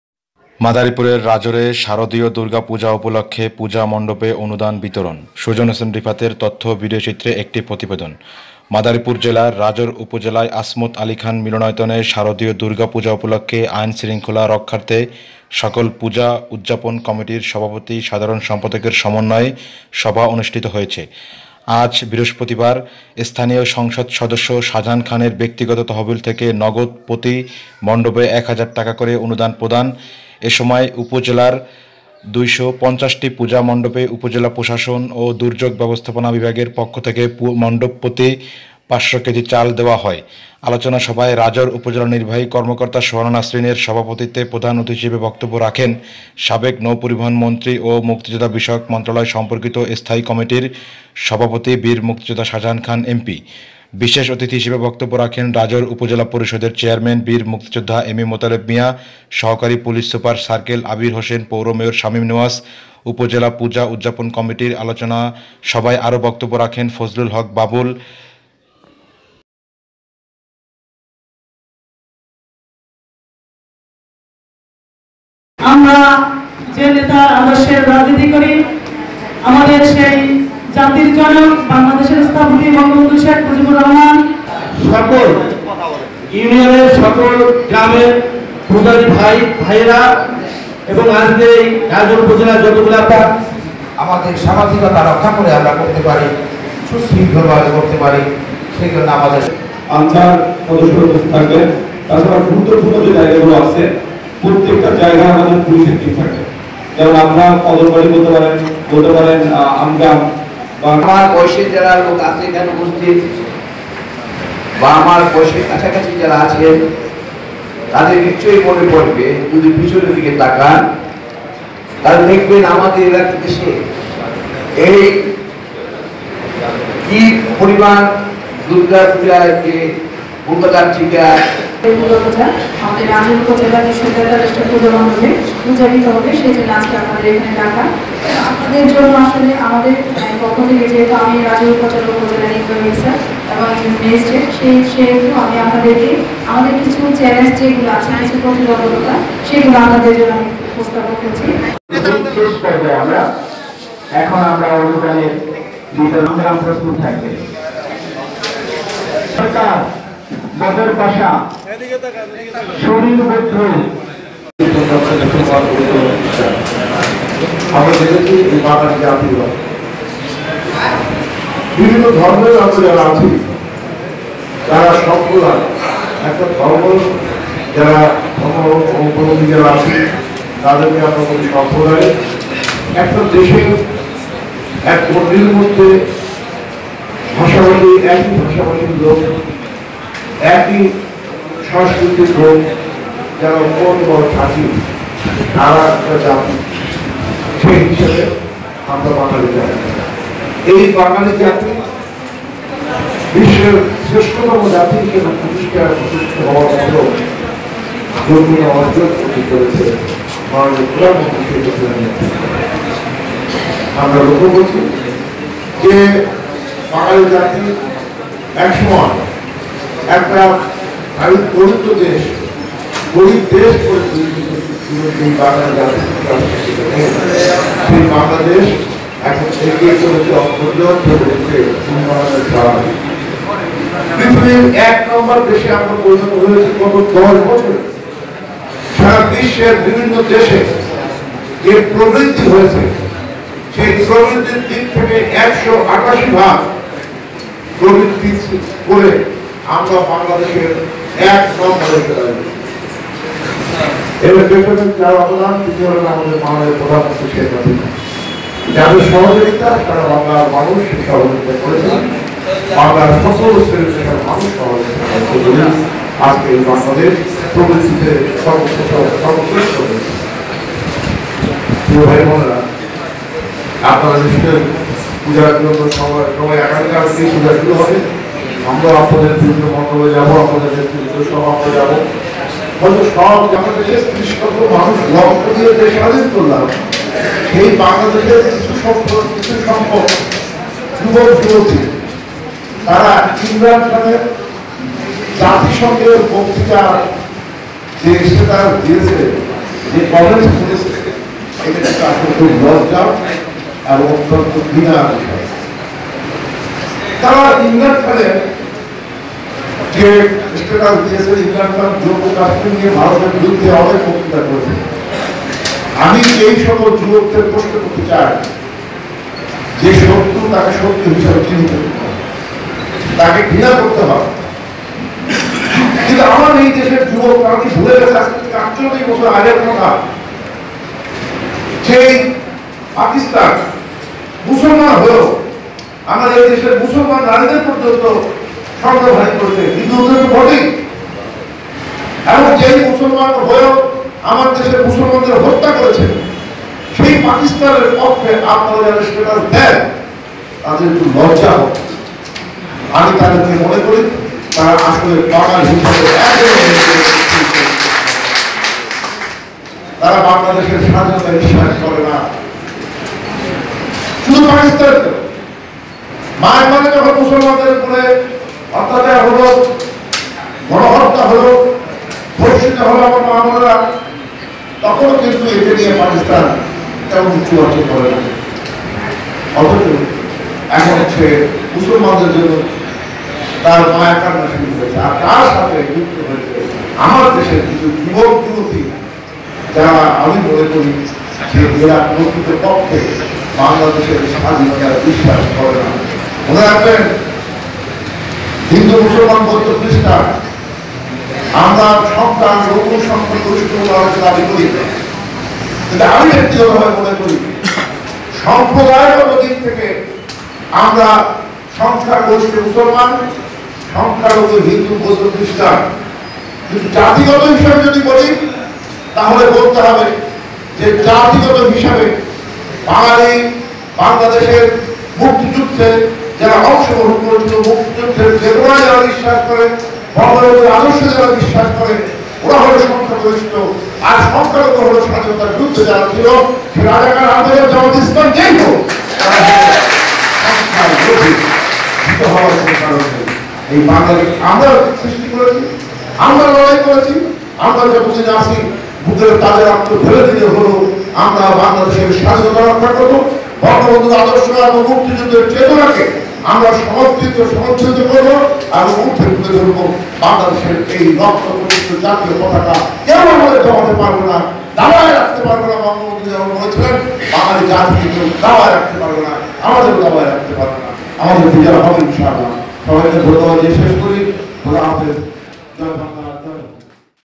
যারা পাকিস্তানিদের পক্ষে মায়া-কান্না করে তারা তারা এদেশের স্বাধীনতায় বিশ্বাস করে না
রাজৈরে শারদীয় দুর্গাপূজা উপলক্ষ্যে পূজা মন্ডপে অনুদান বিতরন

রাজৈর প্রতিনিধি (মাদারীপুর)

মাদারীপুর জেলার রাজৈর উপজেলায় আচমত আলী খান মিলনায়তনে শারদীয় দুর্গাপূজা উপলক্ষ্যে আইন শৃঙ্খলা রক্ষার্থে সকল পুজা উদ্যাপন কমিটির সভাপতি/সাধারন সম্পাদকের সমন্বয়ের সভা অনুষ্ঠিত হয়। বৃহস্পতিবার দুপুরে স্থানীয় সংসদ সদস্য শাজাহান খানের ব্যক্তিগত তহবিল থেকে নগদ প্রতি মন্ডবে ১০০০/- টাকা করে অনুদান রাখেন। এসময় উপজেলার ২৫০ টি পুজা মন্ডপে উপজেলা প্রশাসন ও দুর্যোগ ব্যবস্থাপনা বিভাগের পক্ষ থেকে পূজা মন্ডপ প্রতি ৫০০ কেজি চাল দেওয়া হয়। আলোচনা সভায় রাজৈর উপজেলা নির্বাহী কর্মকর্তা সোহানা নাসরিন এর সভাপতিত্বে প্রধান অতিথির বক্তব্য রাখেন সাবেক নৌ-পরিবহন মন্ত্রী ও মুুক্তিযোদ্ধা বিষয়ক সর্ম্পকিত স্থায়ী কমিটির সভাপতি বীরমুক্তিযোদ্ধা শাজাহান খান এমপি। বিশেষ অতিথি হিসেবে বক্তব্যে রাখেন রাজৈর উপজেলা পরিষদের চেয়ারম্যান বীরমুক্তিযোদ্ধা এম. এম মোতালেব মিয়া, সহকারী পুলিশ সুপার (সার্কেল) আবির হোসেন, পৌর মেয়র শামীম নেওয়াজ। উপজেলা পুজা উদ্যাপন কমিটির আলোচনা সভায় আরো বক্তব্যে রাখেন ফজলুল হক বাবুল ভাইস চেয়ারম্যান রাজৈর উপজেলা পরিষদ, সেলিনা আক্তার মহিলা ভাইস চেয়ারম্যান রাজৈর উপজেলা পরিষদ, পুজা উদ্যাপন কমিটির সহ-সভাপতি বিনোদ রঞ্জন গাইন, সাধারন সম্পাদক মনি সুশিল কুমার সরকার, কদমবাড়ী ইউপি চেয়ারম্যান বিধান বিশ্বাস এসময় আরো উপস্থিত ছিলেন শাজাহান মিয়া অফিসার ইনচার্জ, রাজৈর থানা কবিরাজপুর ইউনিয়ন পরিষদের চেয়ারম্যান আলহাজ্ব টিপু সুলতান মাতুব্বর, হামিদুল শাহ আলম চেয়ারম্যান খালিয়া ইউনিয়ন, সাহাবুদ্দিন সাহা সভাপতি জাতীয় শ্রমিক লীগ রাজৈর উপজেলা, পল্লবী হাসান সাবেক ভাইস চেয়ারম্যান, মরিয়ম মুজাহিদা অধ্যক্ষ সরকারি রাজৈর ডিগ্রি কলেজ,সুজন হোসেন রিফাত সদস্য মাদারীপুর জেলা ছাত্রলীগ, এসময় আরো উপস্থিত ছিলেন উপজেলার বিভিন্ন দপ্তরের কর্মকর্তা, সুশিল সমাজের প্রতিনিধি, গনমাধ্যম কর্মী পূজারীবৃন্দরা উপস্থিত ছিলেন।

সুজন হোসেন রিফাত।
মোবা-০১৭৩৫৮০৮২৬২
তারিখ : ০৩-১০-২০১৯ইং